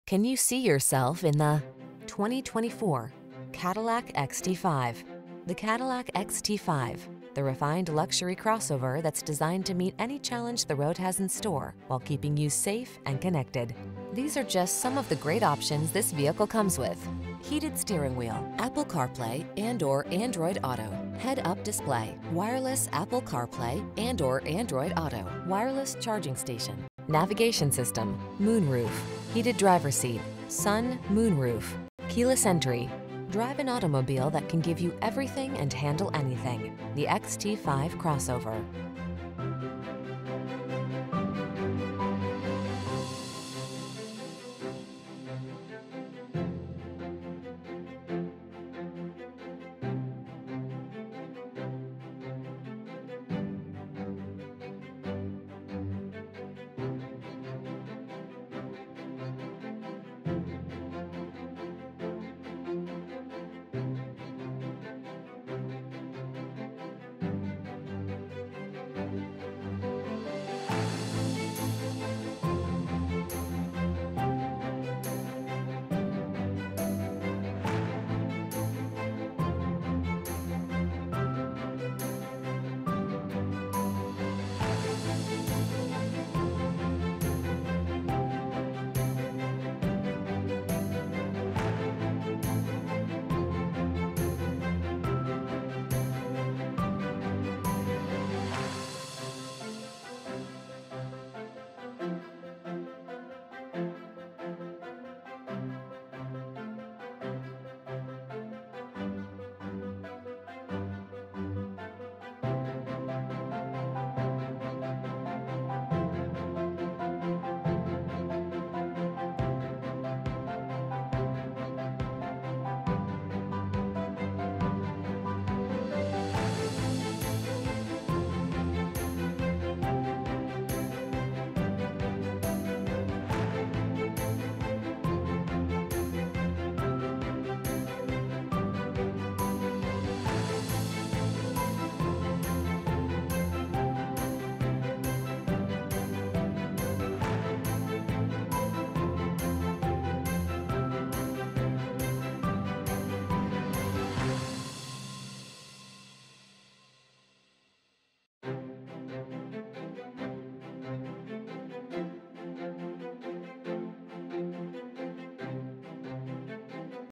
2024 Cadillac XT5 Premium Luxury IL Schaumburg, Arlington Heights, Oakbrook, Hoffman Estates, B...
New 2024 Cadillac XT5 Premium Luxury available at Patrick Cadillac located in 526 Mall Drive, Schaumburg, IL, 60173. Servicing the Schaumburg, Arlington Heights, Oakbrook, Hoffman Estates, Bloomington area.

Can you see yourself in the  2024  Cadillac XT5. The Cadillac XT5: The refined luxury crossover that's designed to meet any challenge the road has in store while keeping you safe and connected. These are just some of the great options this vehicle comes with: Heated Steering Wheel
, Apple Carplay®/Android Auto®, Head-Up Display, Wireless Apple Carplay®/Android Auto®, Wireless Charging Station, Navigation System, Moonroof, Heated Driver Seat, Sun/Moonroof, Keyless Entry. Drive an automobile that can give you everything and handle anything: the XT5 Crossover.